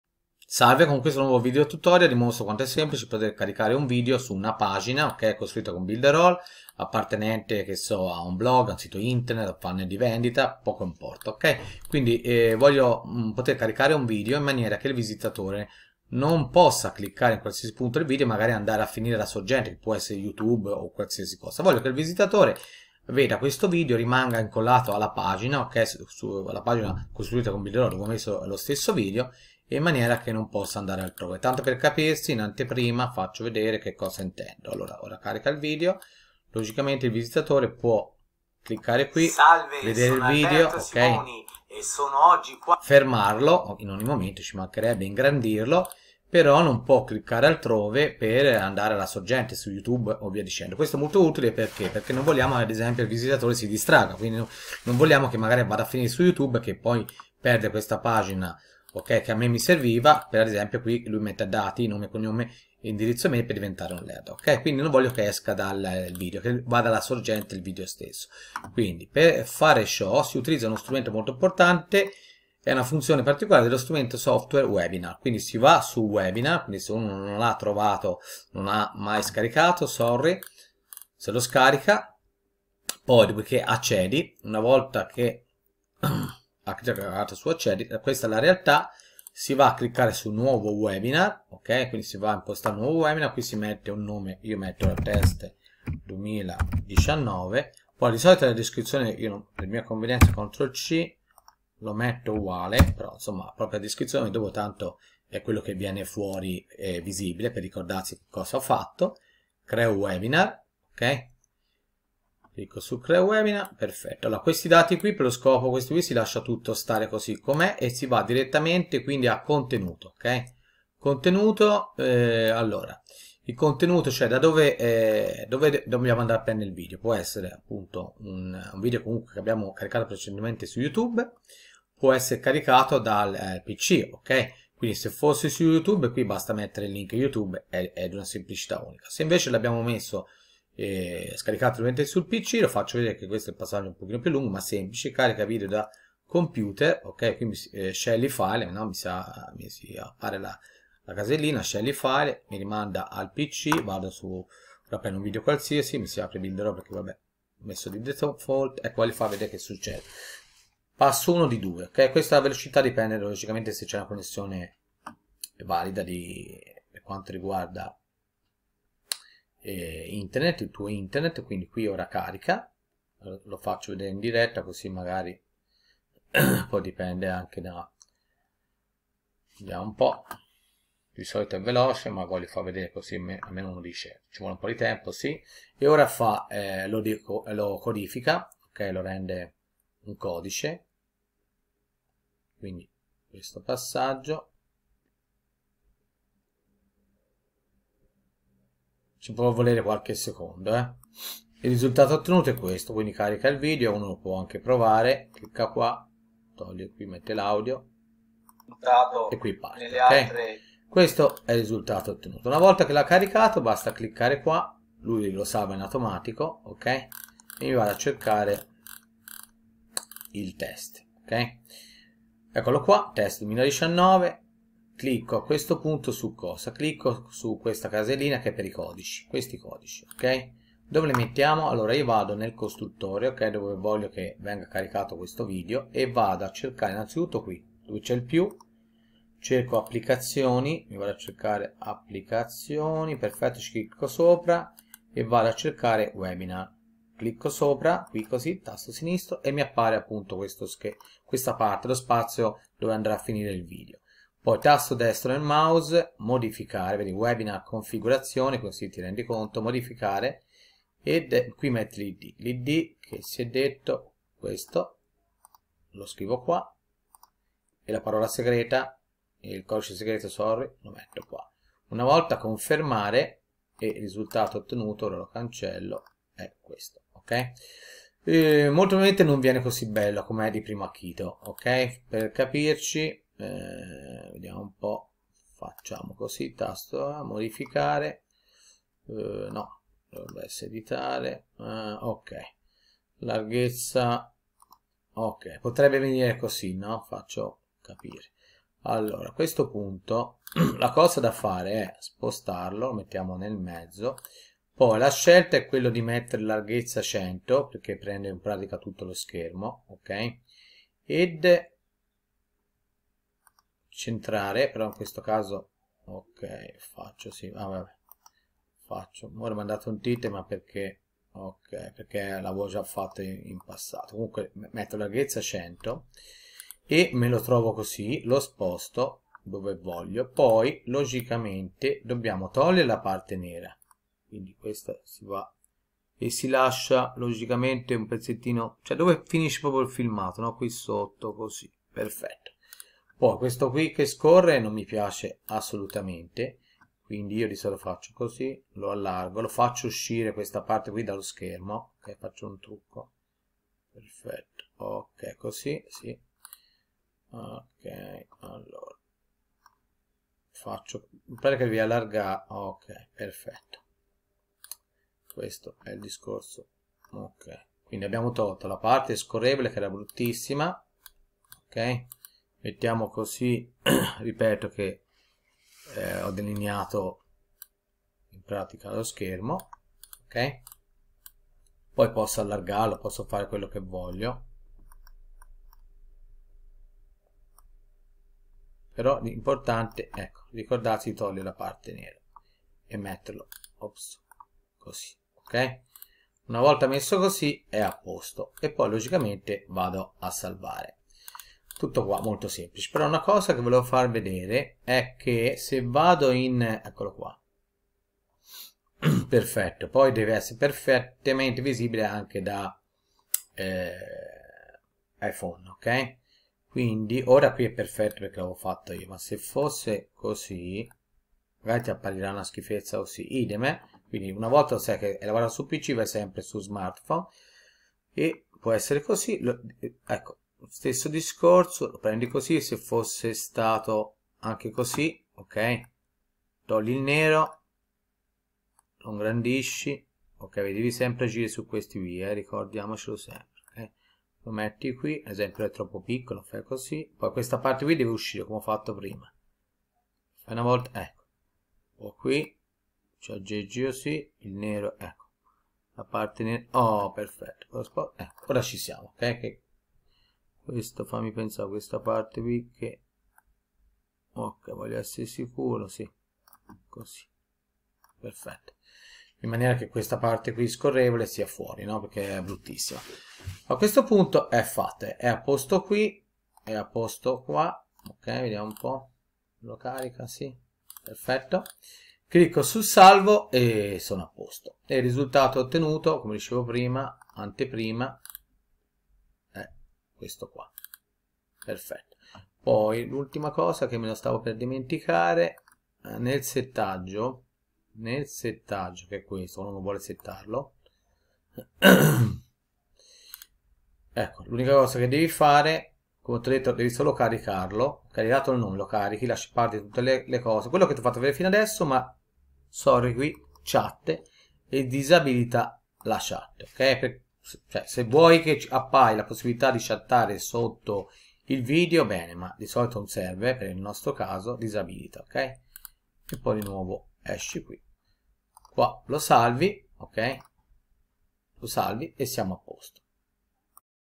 Builderall strumento webinar - ovvero fissare un video su di una pagina
Apriti un account di prova gratuita di tutti gli strumenti di marketing digitale inclusi in Builderall direttamente da qua